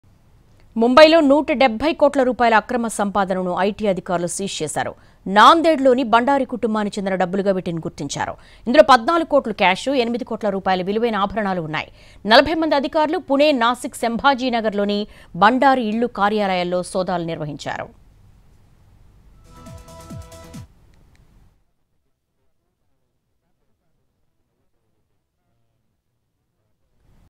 Mumbai: IT Raids In Nanded, Seizes Unaccounted Assets Worth Rs.170 Crore | NTV
For more latest updates on the news :
Mumbai: IT Raid In Nanded, Seizes Unaccounted Assets Worth Rs.170 Crore | NTV